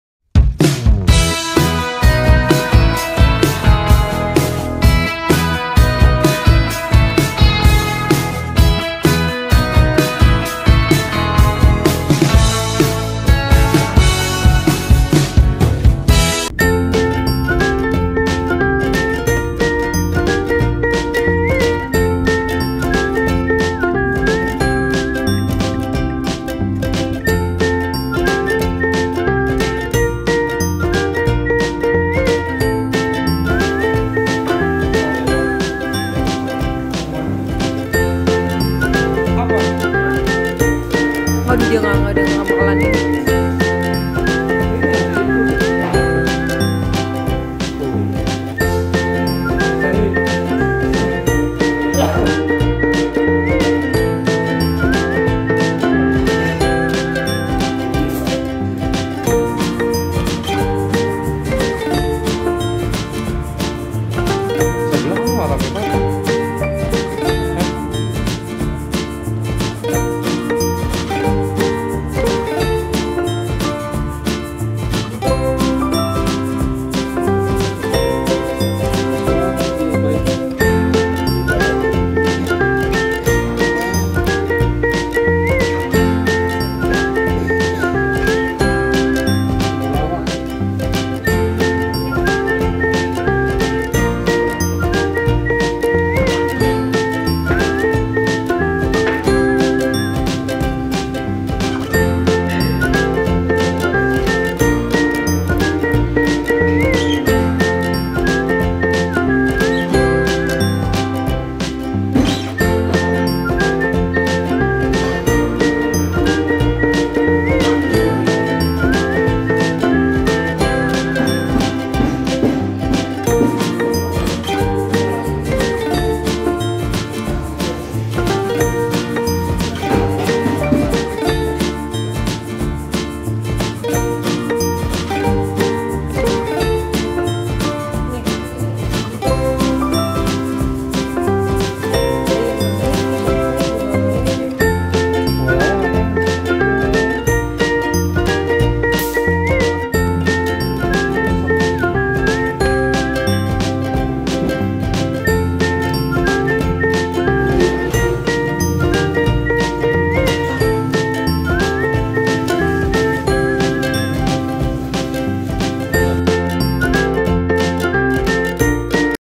Nongkrong di cafe yg menyehatkan (JSR)
Opiz datang ke cafe milik seorang Dokter Herbal terkenal yg menjual minuman dari rempah rempah